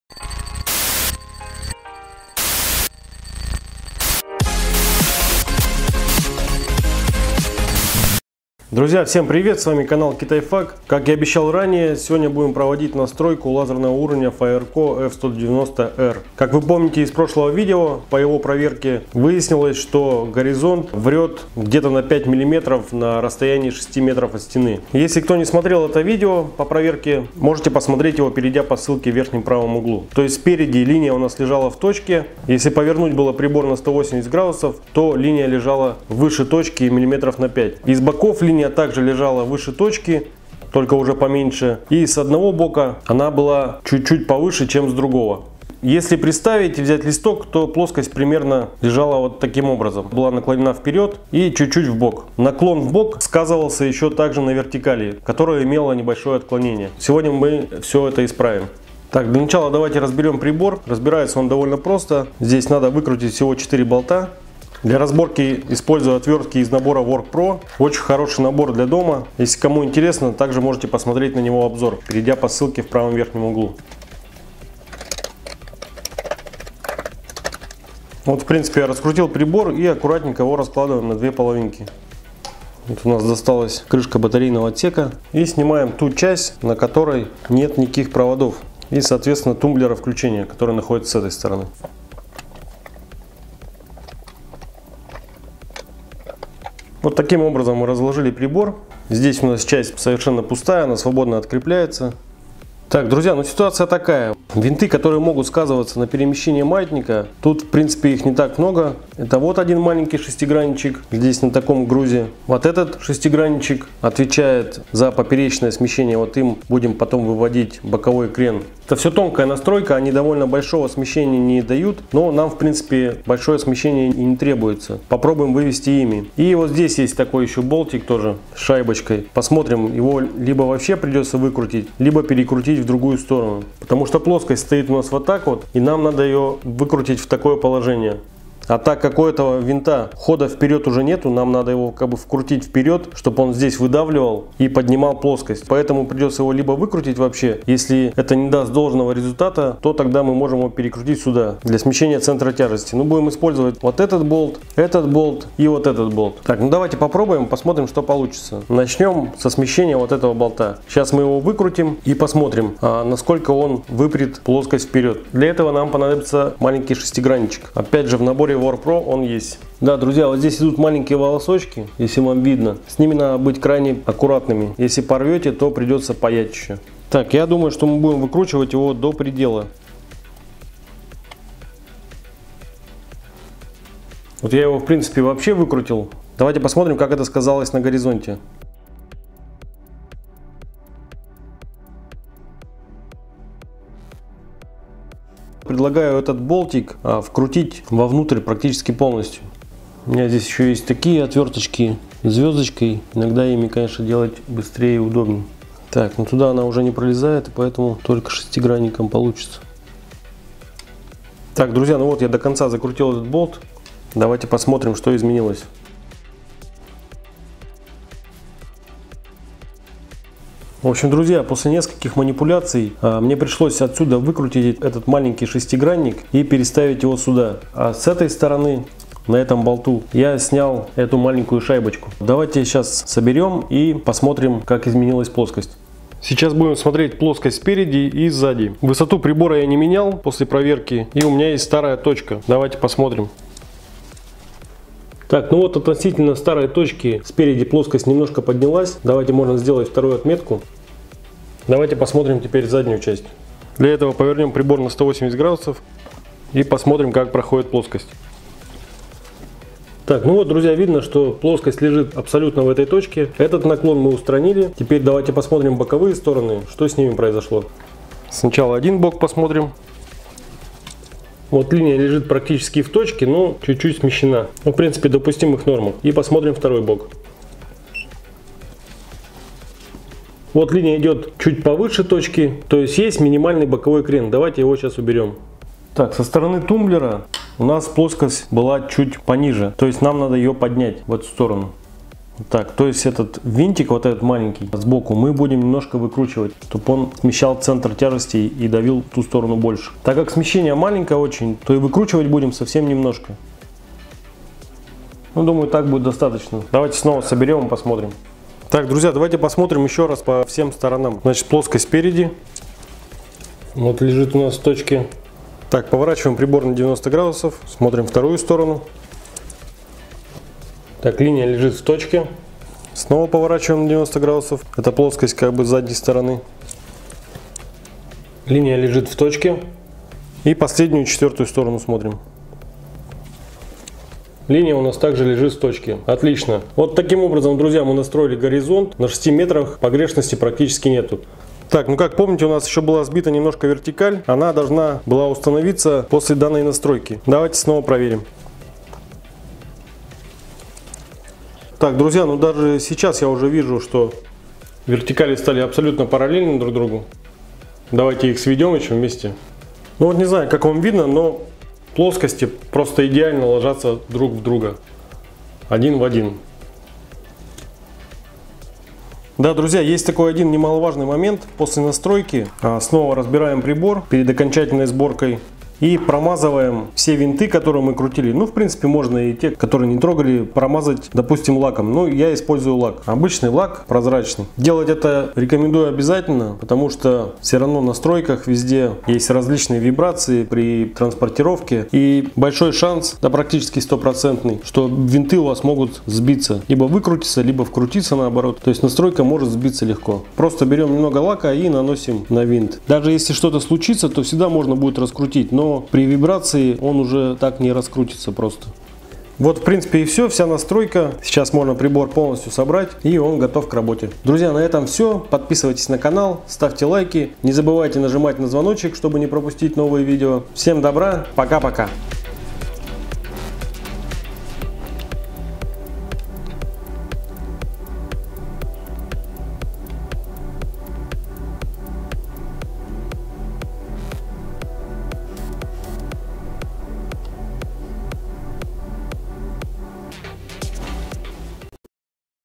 Настройка лазерного уровня Firecore F190R с Алиэкспресс своими руками
Если нужна ссылка на официального продавца смотрите её в моём телеграм канале в этом посте
Проверка лазерного уровня Firecore F190R на точность показала, что уровень имеет погрешность больше, чем заявлено производителем, но это не беда!

В этом видео я разберу и настрою этот лазерный уровень до практически идеальных показателей. Ролик по юстировке очень сложный в съёмке, и если снимать абсолютно всё, то он получится очень долгим и нудным, поэтому я постарался смонтировать самую основную выжимку по регулировке лазерного уровня firecore f190r.

Друзья, все настройки лазерных нивелиров по большому счёт очень схожи, вне зависимости от моделей, главное правильно понять куда наклонена плоскость, и уже тогда потихоньку можно выправить её положение.

Так что если вдруг Вам попался лазерный уровень, который немного врёт, то не отчаиваетесь, так как под силу каждому сделать настройку лазерного уровня своими руками, и самое главное, если подойти к вопросу скрупулёзно, то можно настроить уровень в разы! точнее, чем завод изготовитель!